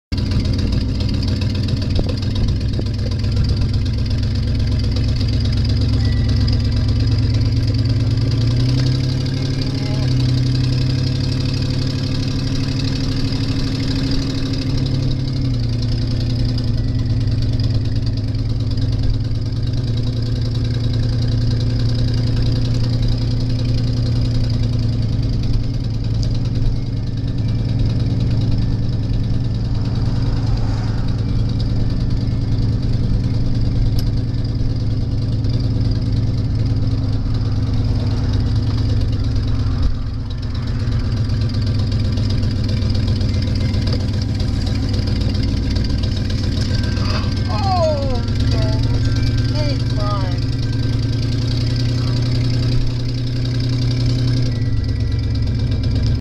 valdez alaska downtown
a short tour of our town. ( i think the rv sounds like a harley.  manifold on order.)
as you can see...the mts are 360...there's a huge bay within feet of these views. 0 stoplights. several stop signs. one grocery store. huge oil pipeline terminal "across the bay". y'know...the usual.